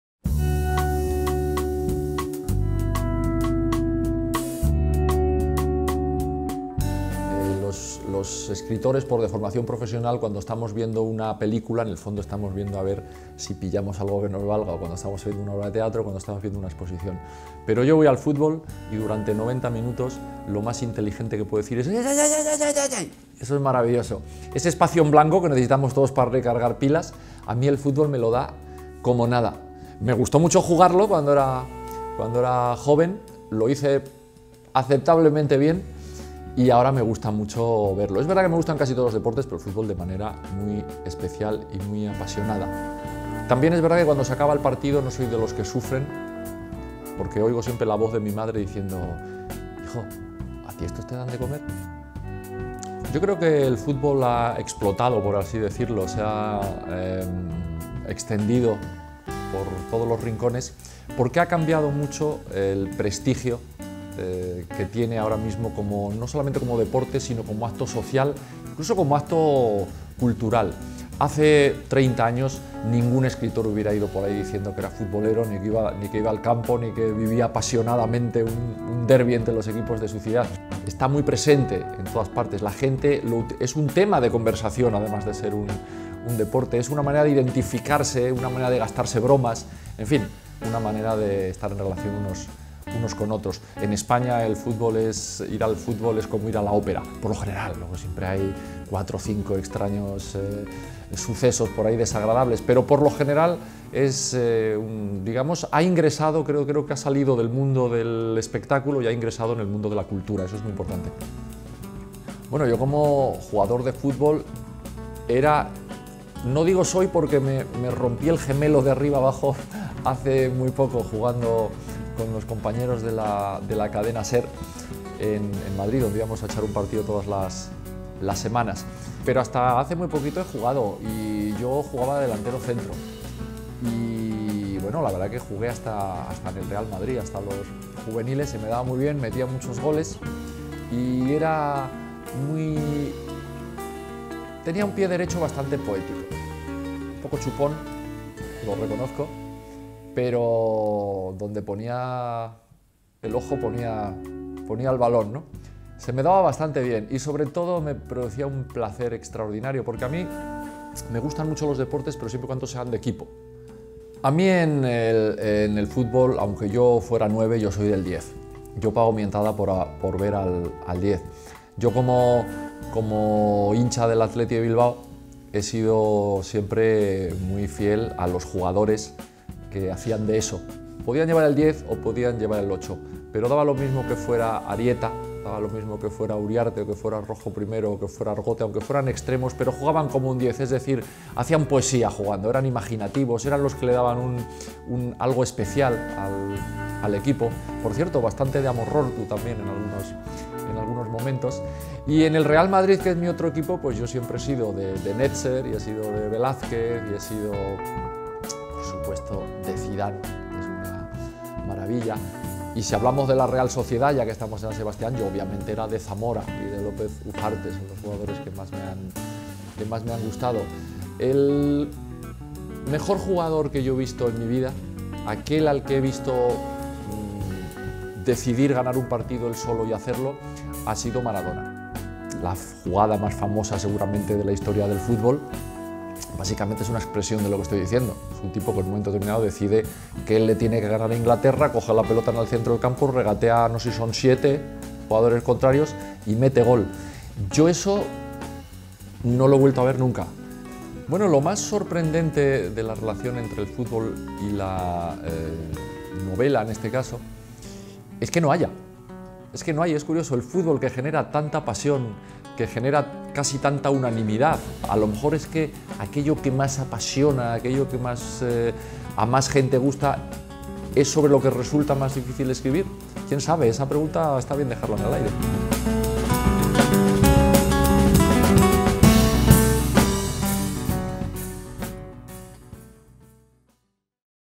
Cuestionario Korner: Benjamín Prado
El escritor y poeta Benjamín Prado nos cuenta sus vivencias entorno al fútbol.

Benjamín Prado es un escritor y poeta español, aficionado al fútbol.  El fútbol es un espacio en blanco necesario. Reflexiones entorno a la evolución del futbol.  Benjamín ha sido delantero centro de los juveniles del Real Madrid, aunque es hincha del Athletic de Bilbao. Zidan, Zamora, López Ufarte, Maradona... La semifinal contra Inglaterra.

Korner es un festival organizado por Donostia Kultura y la Real Sociedad que aúna cultura y fútbol.